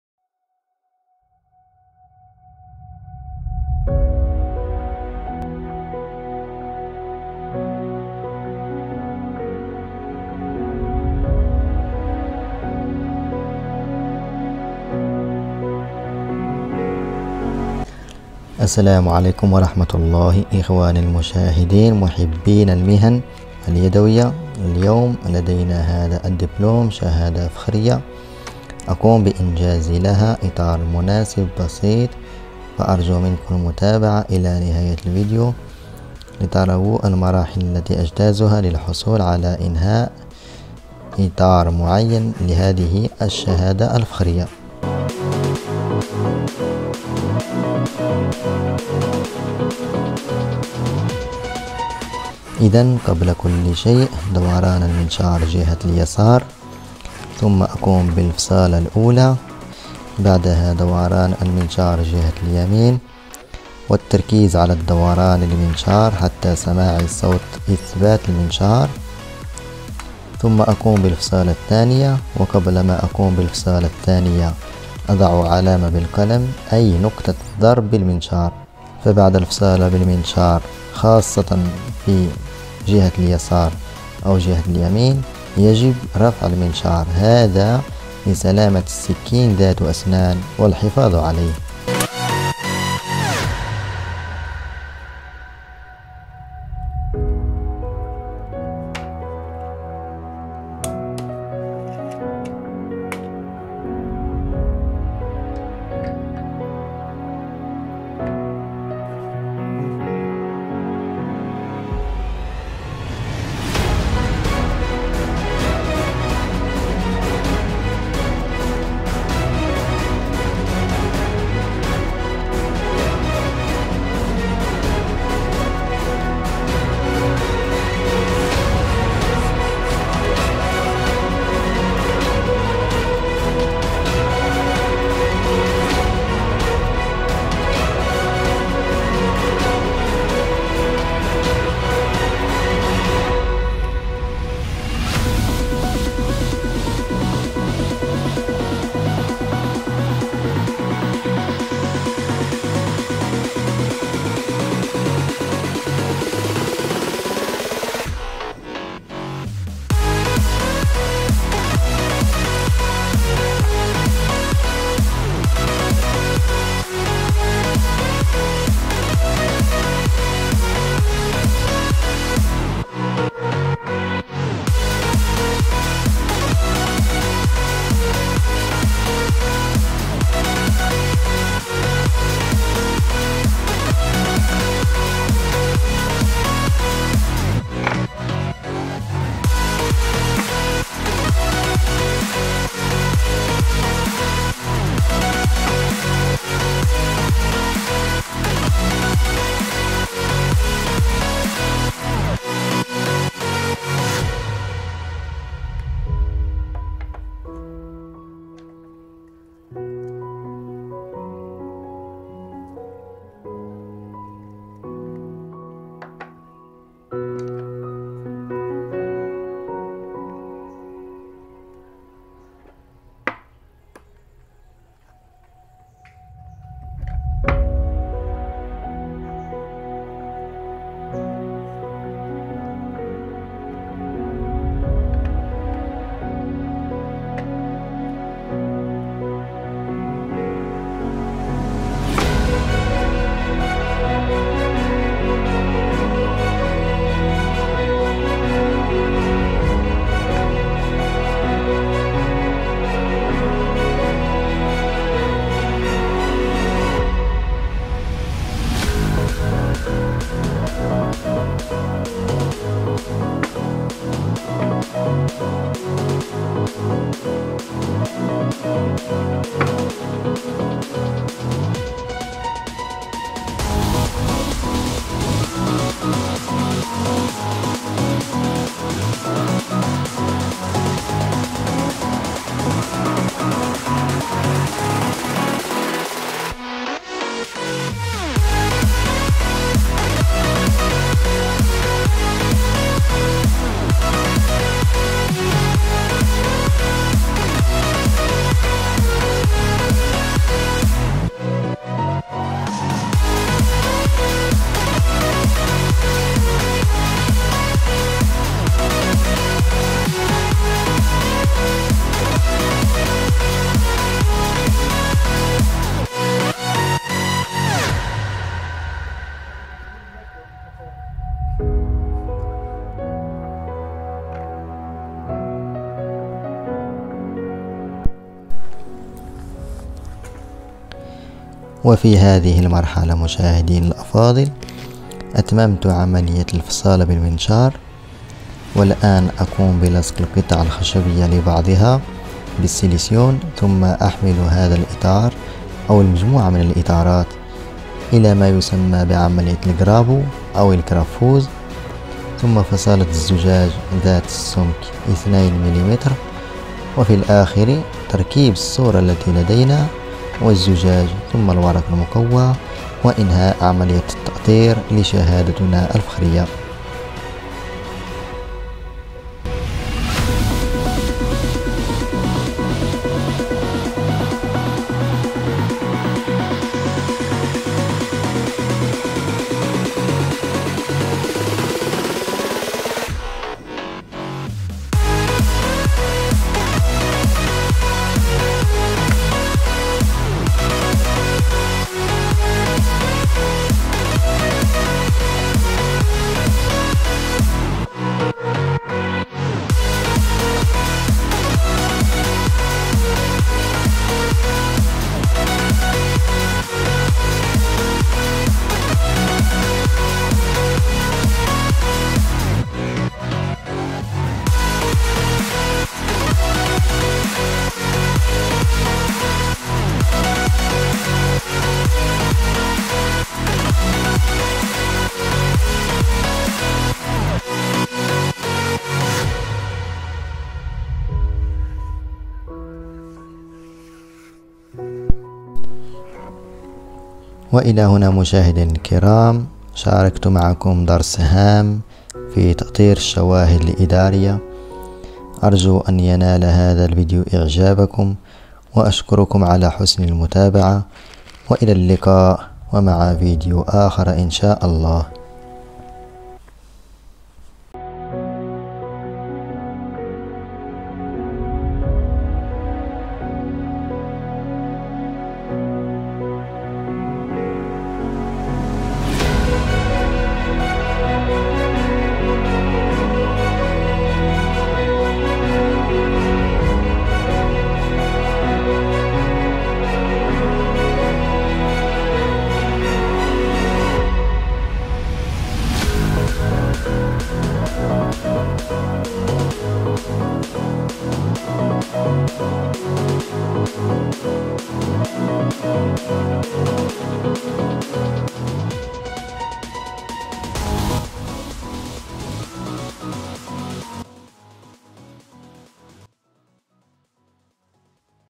شرح طريقة عمل اطار لشهادة فخرية 🖼📋 Expliquer comment faire un cadre pour un certificat
السلام عليكم اصحاب قناة معدات صغيرة وبسيطة درس جديد لنتعلم فيه كيفيه وضع اطار للشواهد الإداريةQue la paix soit sur vous, propriétaires d'une chaîne d'équipement petit et simple, une nouvelle leçon dans laquelle nous apprenons à définir un cadre pour les preuves administratives